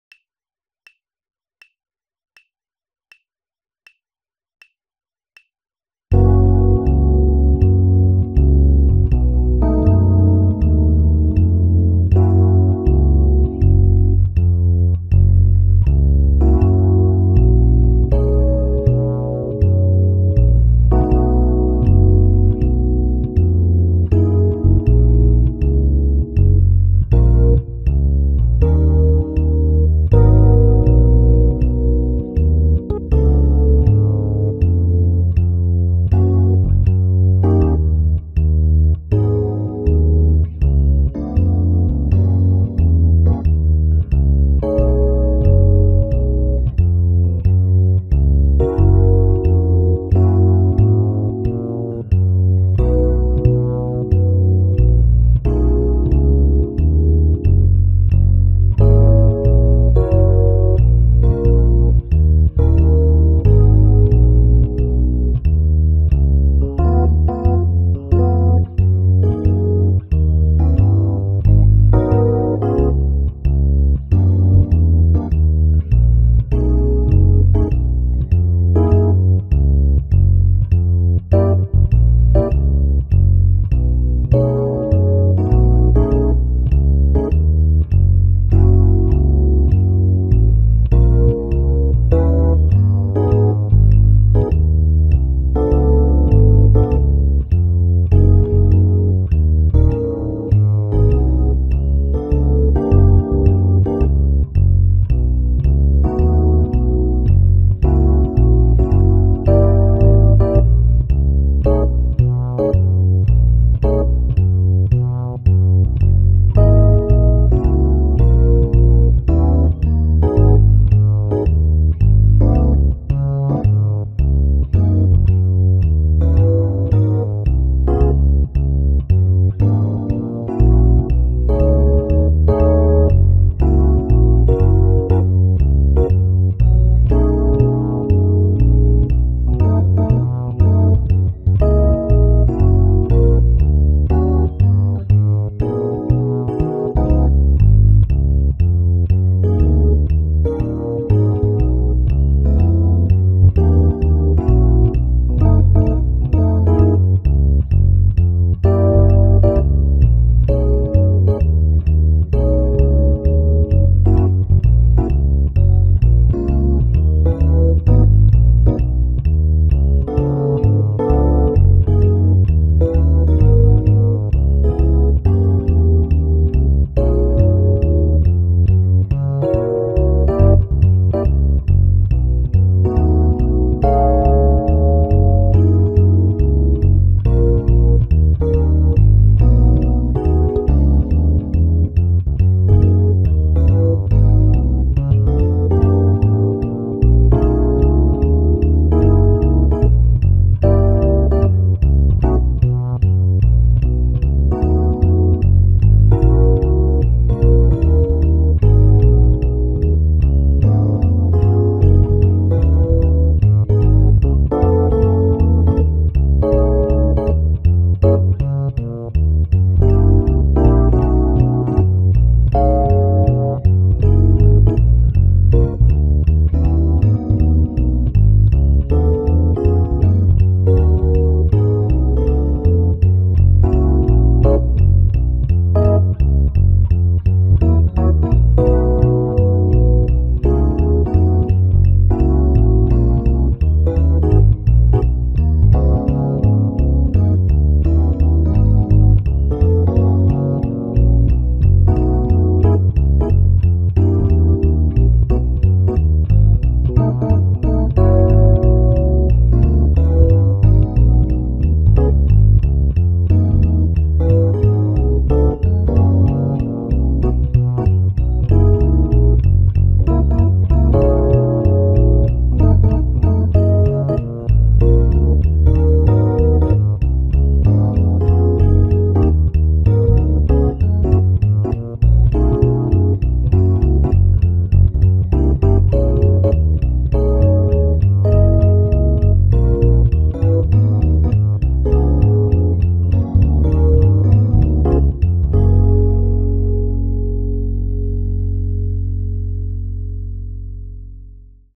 Gb Jazz Blues / Backing Track For Drum (Accelerating Tempo 80-200) Swing Style
0:00 BPM 80
1:14 BPM 100
2:09 BPM 120
2:55 BPM 140
3:35 BPM 160
4:10 BPM 180